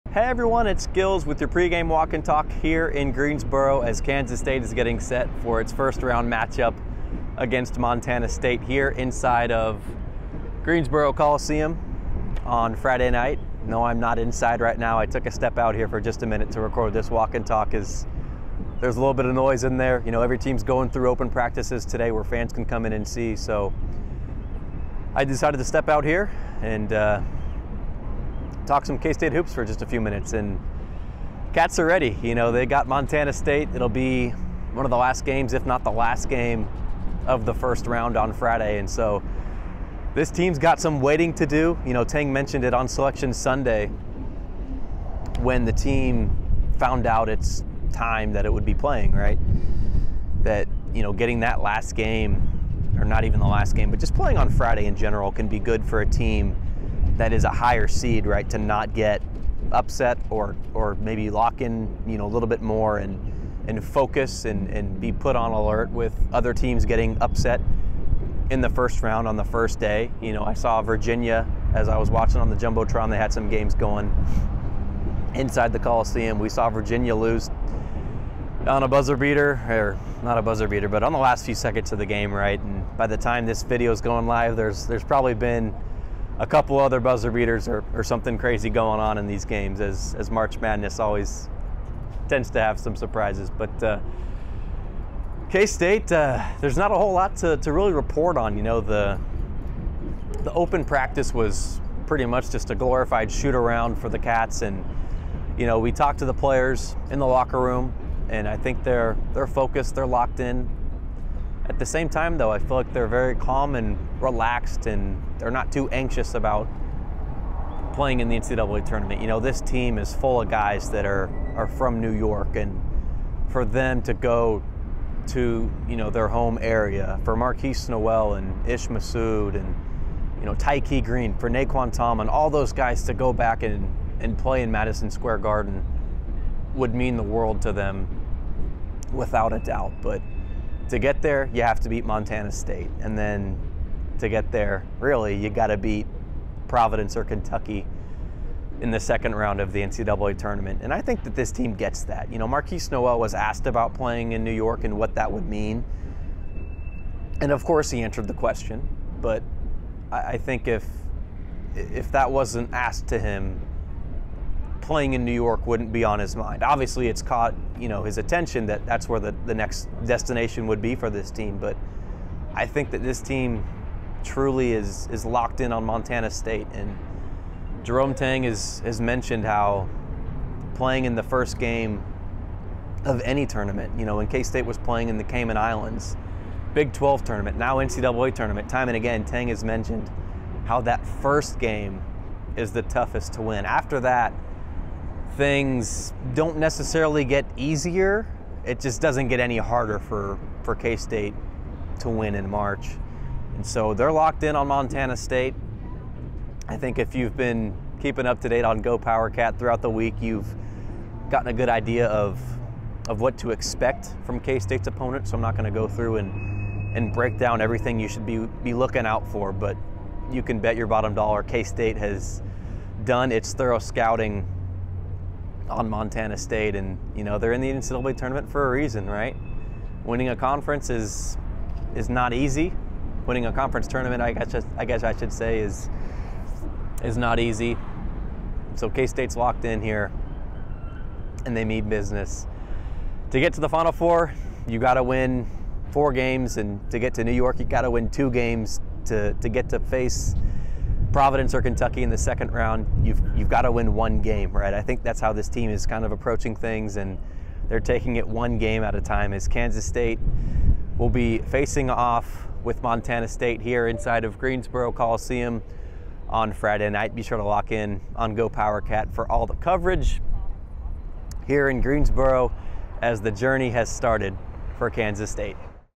Pregame Walk & Talk | Gilz previews Kansas State's First Round game against Montana State
GoPowercat's Ryan Gilbert discusses Kansas State's upcoming game against Montana State in the First Round of the 2023 NCAA Tournament on his Pregame Walk & Talk.